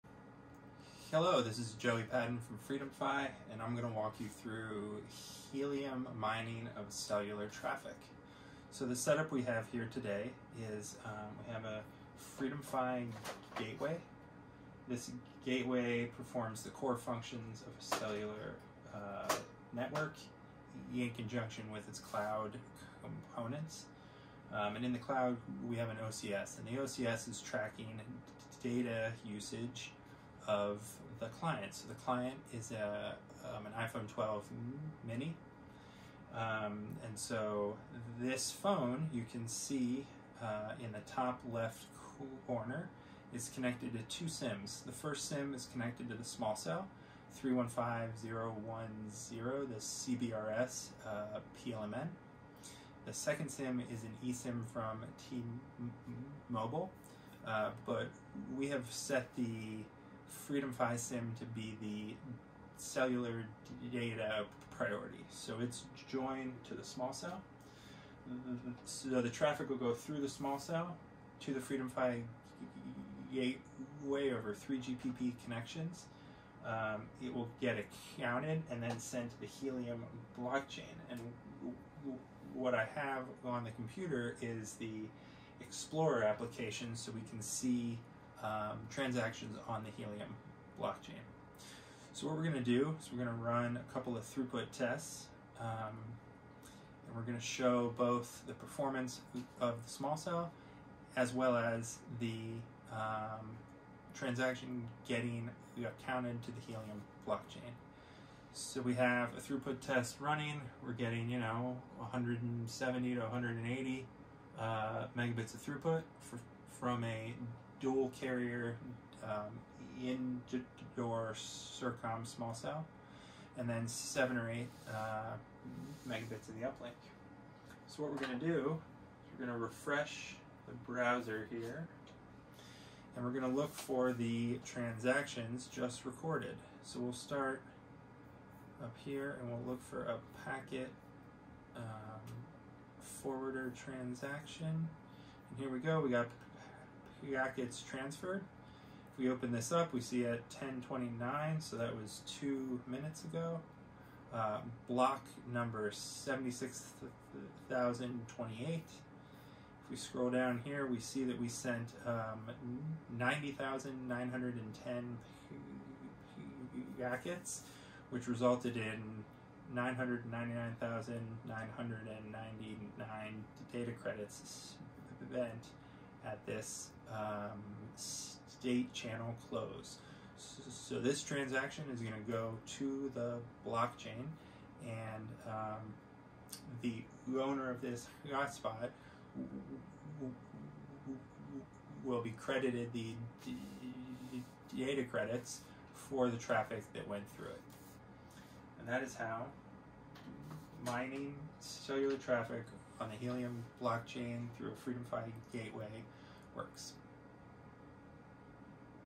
Mining Helium with FreedomFi Gateway
FreedomFi CTO shows off proof of concept mining of cellular traffic on Helium test net using CBRS small cell and iPhon12